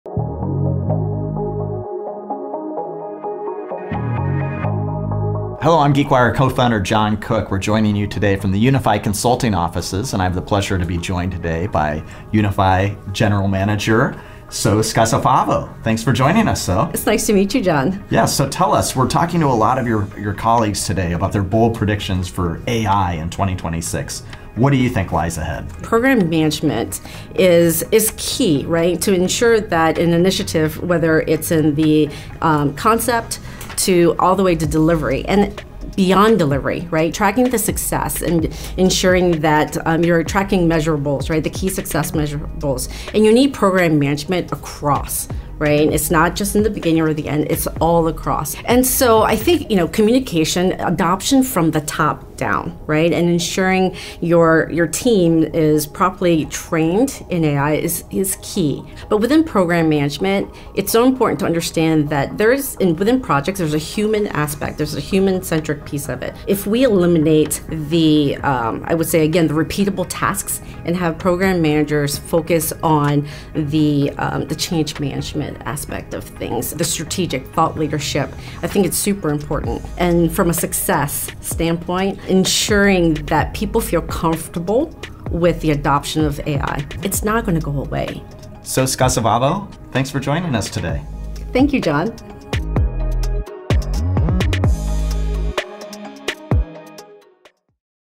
GeekWire Studios: Program management with So Scazafavo
GeekWire Studios | Unify Consulting: 2026 AI Prediction Series

Why is program management going to be integral to AI? Watch this video from Geekwire and the AI experts at Unify Consulting to learn how leaders can make sure complex AI products achieve success.